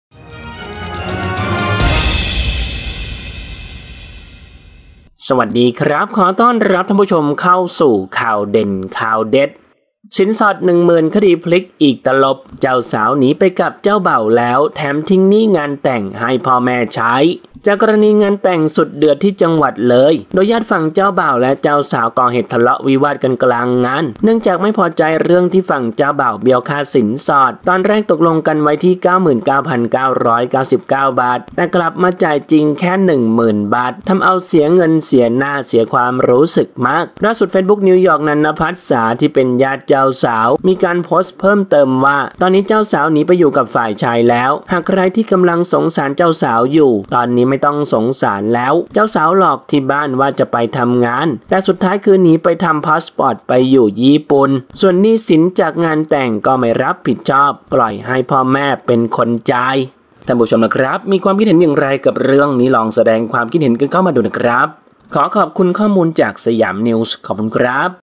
สินสอด 1 หมื่น คดีพลิกอีกตลบ เจ้าสาวหนีไปกับเจ้าบ่าวแล้ว แถมทิ้งหนี้งานแต่งให้พ่อแม่ใช้
ขอบคุณ  เฟซบุ๊ก นิวยอร์ก นันณพัทรศาฯ